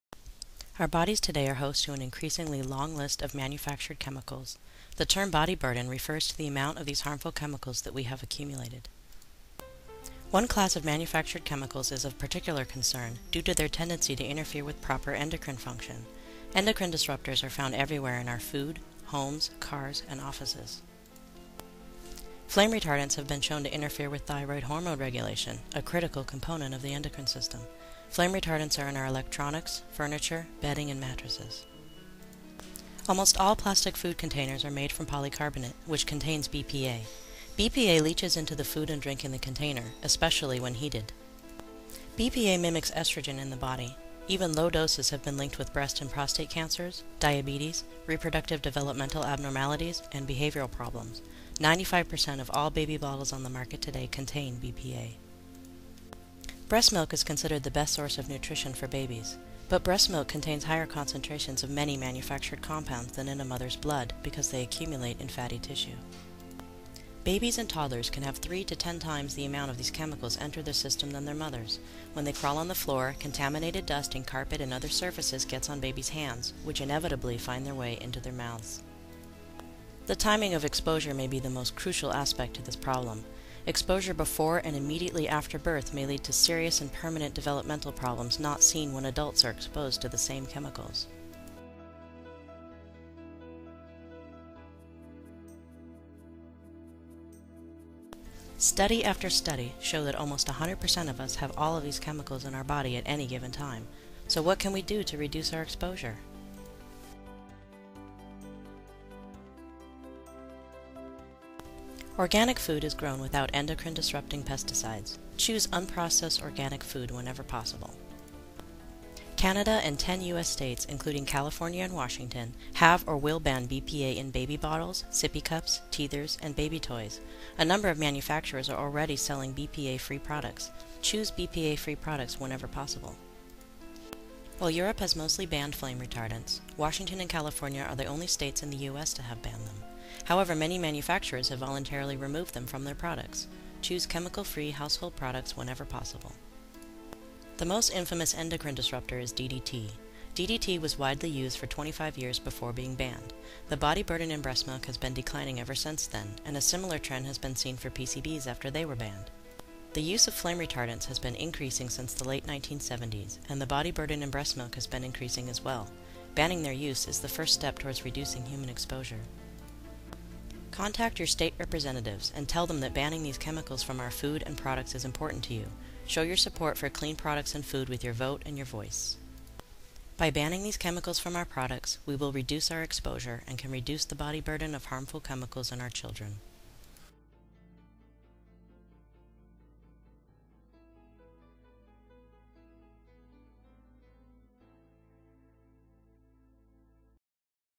Endocrine Disruptors: The Body Burden in Our Babies
Endocrine disruptors are in our homes, cars, offices, and food. Babies and toddlers have higher levels in their systems than adults. Endocrine disruptors interfere with hormone regulation. Exposure during development can cause serious and permanent health problems.